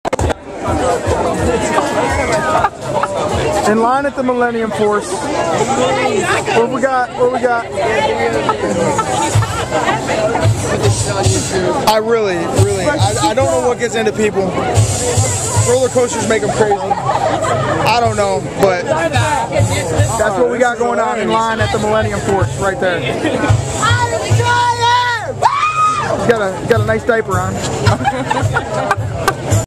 Millenium Force Dancer
So we are all chillin in line to get on the Millenium Force, best coaster ever at Cedar Point in Sandusky Ohio.. and this dude danced around for 45 minutes STRAIGHT... i had no choice but to bust out the flip video and take some footage ...